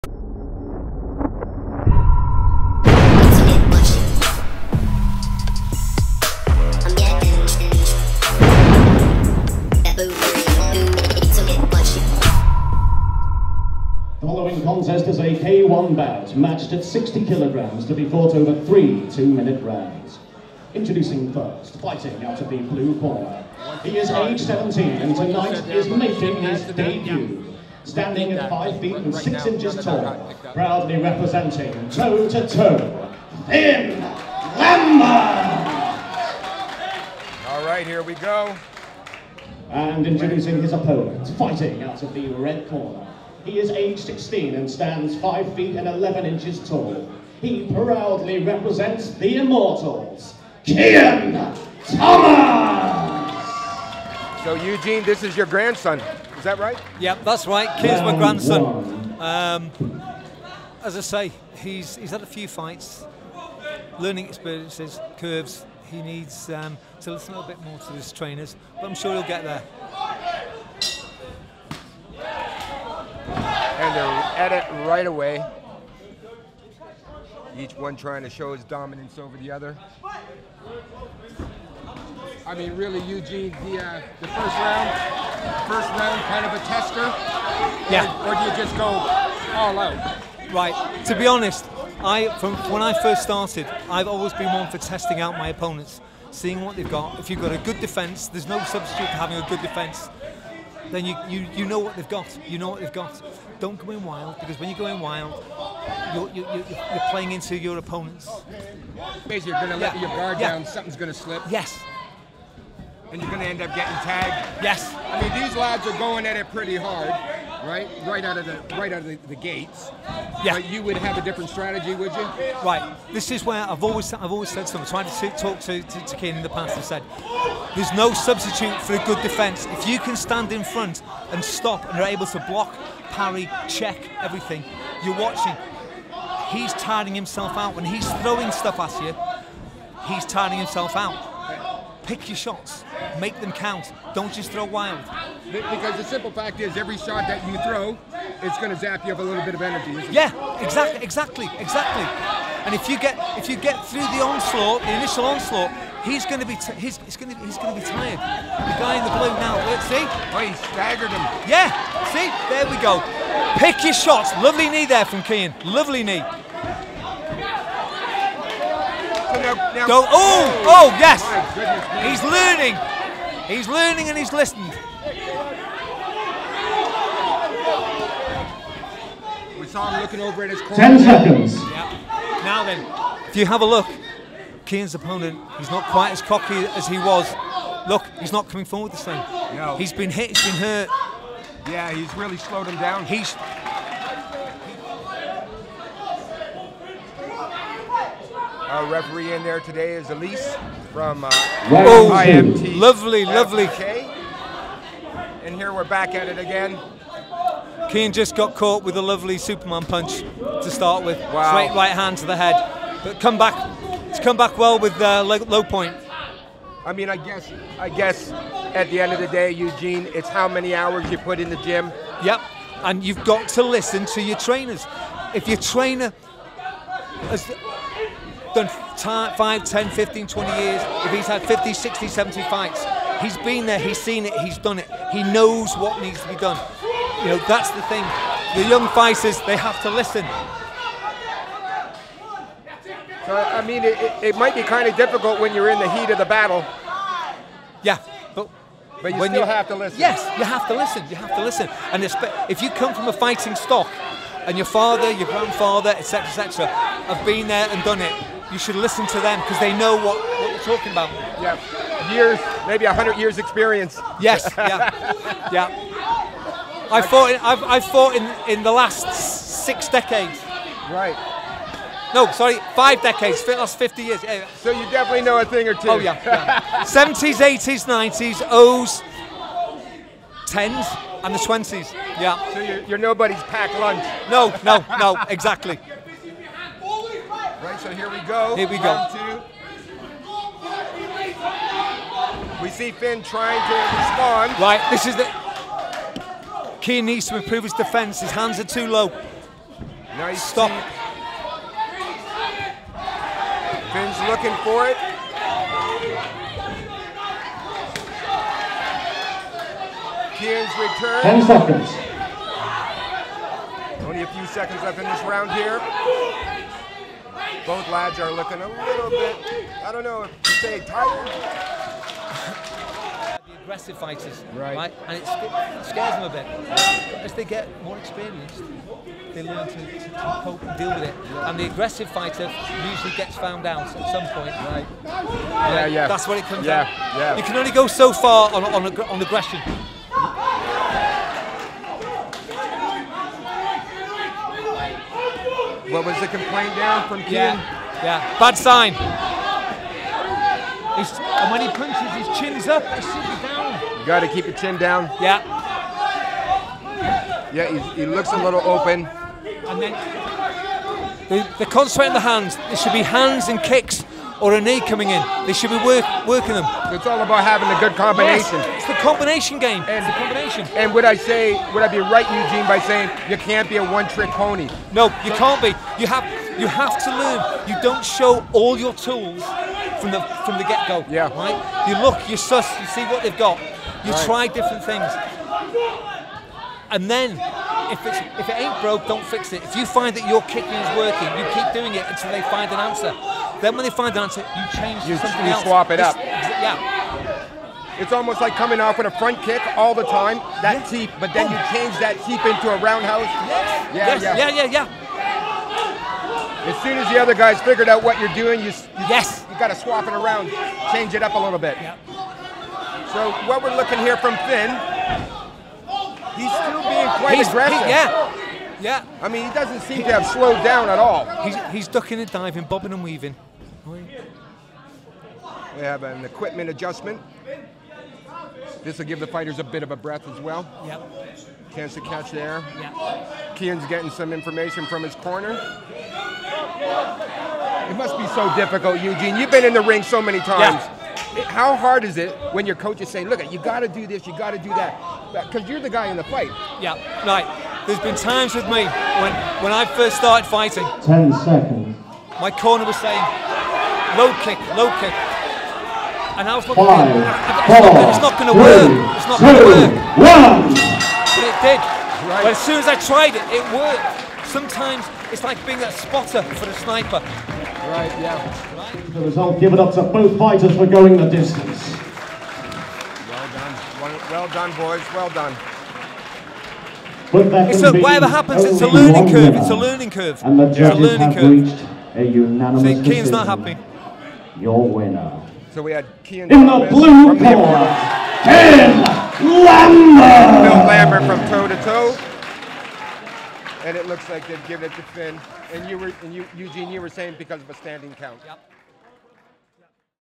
Fin Lambert vs Kian Thomas
Monsters Ball - 28.10.23 Broadway Boulevard, Llandudno

MC Ronnie Rowlands
Commentary by Gary Elder and Eugene Valerio

Shot & Edited by Robin Abas